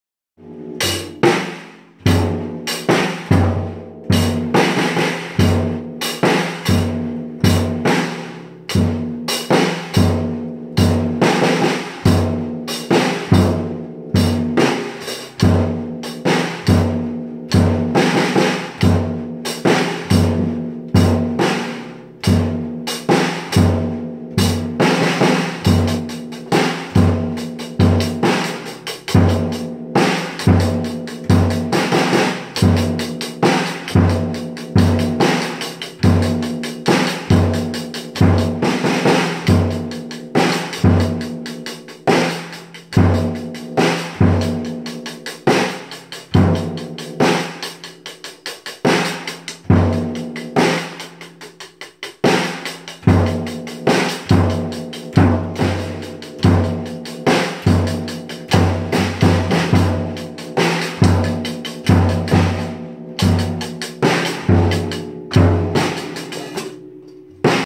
Beginner Drummer
KJ working on his new drum set. Wish I knew someone that gave drummer lessons!